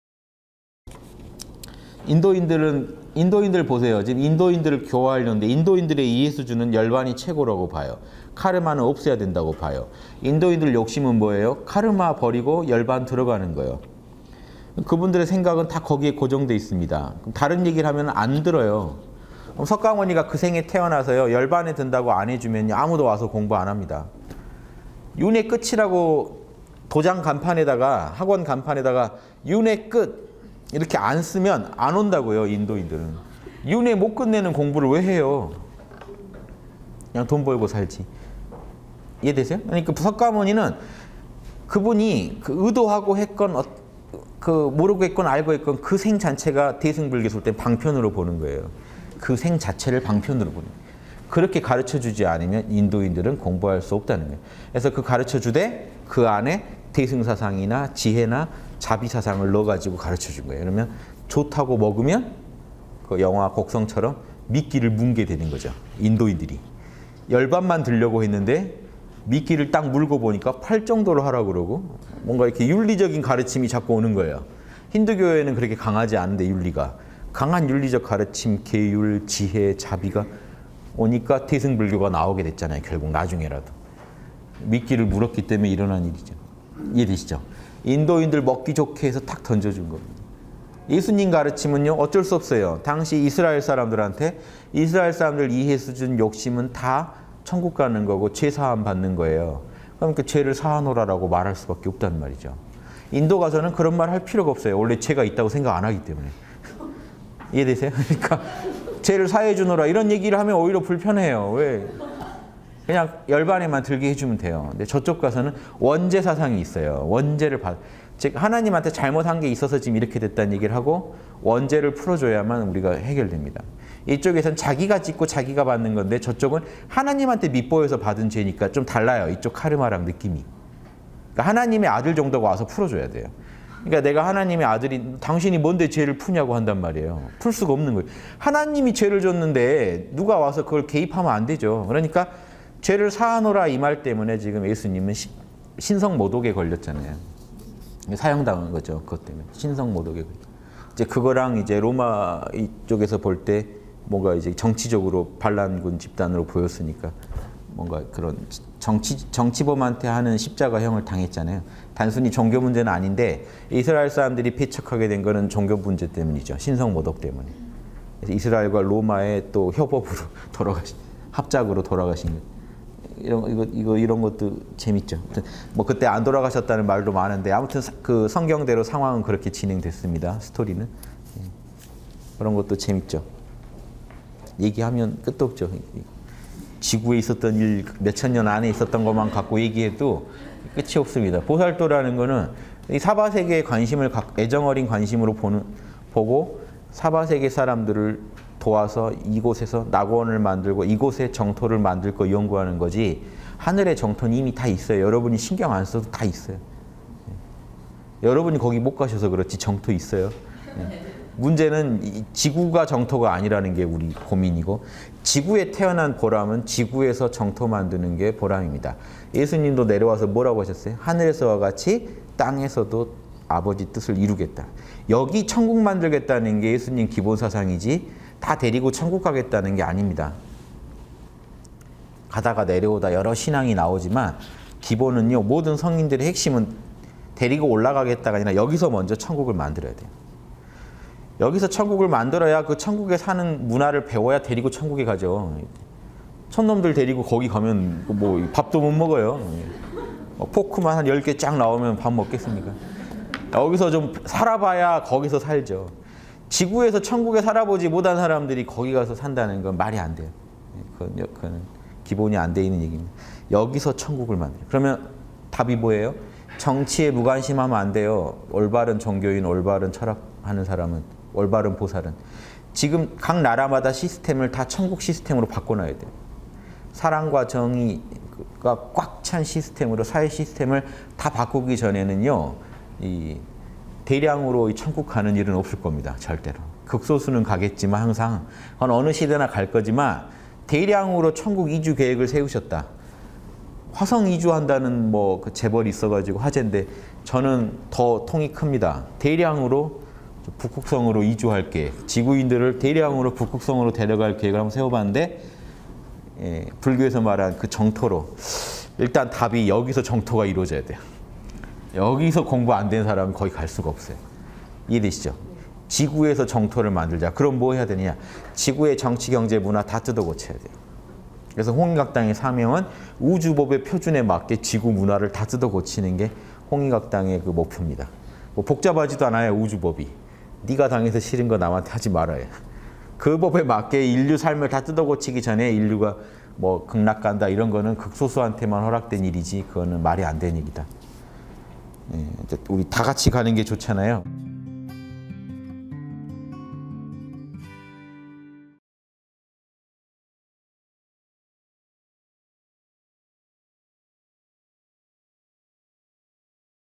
[3분 양심] 성인들의 시대를 초월한 방편 _홍익학당
- (160709 화엄경 강의) 성인들의 시대를 초월한 방편
인도인들 보세요. 인도인들을 교화하려는데,
인도인들의 이해 수준은 열반이 최고라고 봐요.
카르마는 없애야 된다고 봐요 인도인들 욕심은
뭐예요? 카르마 버리고 열반 들어가는 거예요.
그 분들의 생각은 다 거기에 고정돼 있습니다.
다른 얘기를 하면 안 들어요.

석가모니가 그 생에 태어나서 열반에 든다고
안 해주면, 아무도 와서 공부 안 합니다.
윤회의 끝이라고 도장 간판에다가 학원 간판에다가
윤회 끝. 이렇게 안 쓰면 안 온다고요, 인도인들은.
이 윤회 못 끝내는 공부를 왜 해요?
그냥 돈 벌고 살지. (이런 생각이) 이해되세요?

그러니까 석가모니는 그 분이 의도하고 했건,
모르고했거나 알고 했건, 그 생 자체가
대승불교 (관점)에서는 방편으로 보는 거예요.
그 생 자체를 방편으로 그렇게 (보는 거예요.)그렇게 가르쳐주지 않으면 인도인들은 공부할 수
없다는 (거에요.) 그래서 가르쳐주되,
그 안에 대승사상이나 지혜나 자비사상을
넣어가지고 가르쳐주고요. 그러면 좋다고 먹으면,

그 영화, 곡성처럼 미끼를 물게 되는 거죠.
인도인들이 열반만 들려고 했는데,
미끼를 딱 물고 보니까 팔 정도로 하라고 그러고
뭔가 이렇게 윤리적인 가르침이 자꾸 오는 거예요.
힌두교회는 그렇게 강하지 않은데, 윤리가.
강한 윤리적 가르침, 계율, 지혜,․자비가 오니까
대승불교가 나오게 됐잖아요, 결국 나중에라도.
미끼를 물었기 때문에 일어난 일이죠.

이해되시죠? (부처님이) 인도인들 먹기 좋게 해서
탁 던져준 거. 예수님 가르침은요? 어쩔 수 없어요.
당시 이스라엘 사람들한테. 이스라엘 사람들 이해
수준, 욕심은 다 천국 가는 거고 죄사함 받는 거예요.
그러면 "그 죄를 사하노라!"라고 말할 수 밖에 없단
말이죠. 인도 가서는 그런 말 할 필요가 없어요.

원래 죄가 있다고 생각 안 하기 때문에. 이해되세요? 그러니까 "죄를 사해주노라!" 이런 얘기를 하면,
오히려 불편해해요. 그냥 열반에만 들게 해주면돼요. 그런데 저쪽 가서는 '원죄사상'이 있어요, 원죄!
즉 하나님한테 잘못한 게 있어서, 지금 이렇게 됐다는 얘기를 하고 원죄를 풀어줘야만 우리가 해결 됩니다.

이 쪽에서는 자기가 (죄를) 짓고 자기가 받는 건데,
저 쪽은 하나님한테 밉보여서 받은 죄니까 좀 달라요.
이 쪽 카르마랑 느낌이. 하나님의 아들 정도가 와서,
(죄를) 풀어줘야 돼요 그러니까 내가 하나님의 아들인
'당신이 뭔데 죄를 풀냐?'고 한단 말이에요.
풀 수가 없는 거예요. 하나님이 죄를 주셨는데,
누가 와서 그걸 개입하면 안 되죠. 그러니까
"죄를 사해주노라!". 이 말 때문에. 지금 예수님은
신성 모독에 걸렸잖아요. 사형당하신 거죠, 그것
때문에. 신성 모독(과 함께) 이제 로마 쪽에서 볼 때,

정치적으로 반란군 집단으로 보였으니까. 뭔가,
그런 정치범한테 하는 십자가형을 당하셨잖아요?
단순히 종교 문제는 아닌데, 이스라엘 사람들이
배척하게 된 건 종교 문제 때문이죠.
신성 모독 때문에, 이스라엘과 로마의 또 협업으로
돌아가신 합작으로 돌아가신 이런 것도 재밌죠.
그때 안 돌아가셨다는 말도 많은데. 아무튼
그 성경대로 상황은 그렇게 진행됐습니다.

스토리는 그런 것도 재밌죠.. 얘기하면 끝도 없죠.
지구에 있었던 일. 몇천 년 안에 있었던 것만 갖고
얘기해도 끝이 없습니다. '보살도'라는 것은.
이 사바 세계를 관심을 애정어린 관심으로 보고,
사바 세계 사람들을 도와서 이곳에서 낙원을 만들고
이곳의 정토를 만들 거 연구하는 거지.

하늘의 정토. 이미 다 있어요. 여러분이 신경
안 써도 다 있어요. 여러분이 거기 못 가셔서 그렇지
(하늘)정토 있어요 문제는 지구가 정토가 아니라는
것이 우리의 고민이고 지구에 태어난 보람은.
지구에서 정토 만드는 것이 보람 입니다.
예수님도 내려와서 뭐라고 하셨어요?
"하늘에서와 같이, 땅에서도 아버지 뜻을 이루겠다!"
여기 천국 만들겠다는 것이, 예수님 기본 사상이지

다 데리고 천국을 가겠다는 것이 아닙니다..
가다가 내려오다 여러 신앙이 나오지만 기본은요.
모든 성인들의 핵심은, 데리고 올라가겠다가 아니고.
여기(지상에)서 먼저 천국을 만들어야 된다!
여기서 천국을 만들어야 그 천국에 사는 문화를
배워야 (비로소) 데리고 천국에 가죠.
촌놈들 데리고 거기 천국 가면 밥도 못 먹어요.
포크만 한 10개 쫙 나오면 밥 먹겠습니까?

여기서 좀 살아봐야 거기서 살죠. 지구에서 천국에
살아보지 못한 사람들이 거기 가서 산다는 건,
말이 안 돼요. 기본이 안 돼 있는 얘기입니다.
여기서 천국을 만들(어야). 그러면 답이 뭐예요?
정치에 무관심하면 안 돼요. 올바른 종교인, 올바른
철학하는 사람은, 올바른 보살은, 지금 각 나라마다
시스템을 다 천국 시스템으로 바꿔놔야 돼요..

사랑과 정의 꽉 찬 시스템으로, 사회 시스템을
다 바꾸기 전에는요. 대량으로 천국 가는 일은
없을 겁니다. 절대로. 극소수는 가겠지만 항상,
어느 시대나 갈 거지만 대량으로 천국 이주 계획을
세우셨다. 화성 이주한다는 재벌이 있어서 화제인데,
저는 더 통이 큽니다. 대량으로 북극성으로 이주할
계획, 지구인들을 대량으로 북극성으로 데려갈
계획을 한번 세워봤는데, 불교에서 말한 그 정토로.
일단 답이 여기(지상에)서 정토가 이루어져야 돼요.

여기서 공부 안 된 사람은 거기 갈 수가 없어요.
이해되시죠? 지구에서 정토를 만들자!
그럼 뭐 해야 되느냐? 지구의 정치 경제 문화
다 뜯어 고쳐야 돼요. 그래서 홍익학당의 사명은
우주법의 표준에 맞게, 지구 문화를 다 뜯어고치는
것이 홍익학당의 목표입니다. 복잡하지도 않아요,

우주법이! "네가 당해서 싫은 거 남한테 하지 말라."
에요. 그 법에 맞게 인류 삶을 다 뜯어고치기 전에,
'인류가 뭐 극락간다' 이런 거는
극소수한테만 허락된 일이지.
(극소수만 천국가는) 그거는 말이 안 되는 일이다.
이제 우리 다 같이 가는 게 좋잖아요.